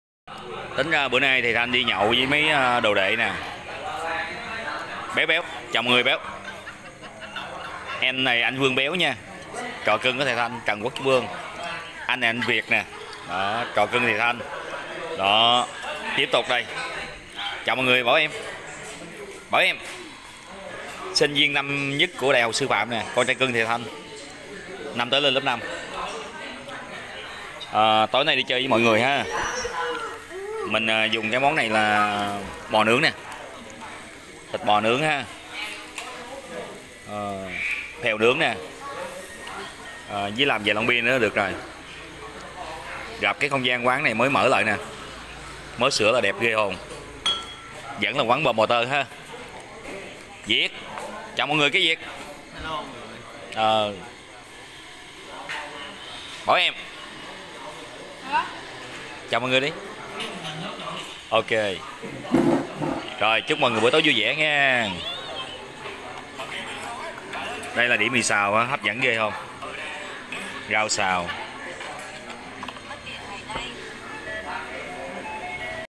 CÁNH BUỒM QUÁN
Lâu lắm mới ghé qua quán đẹp hơn và ăn ngon như ngày nào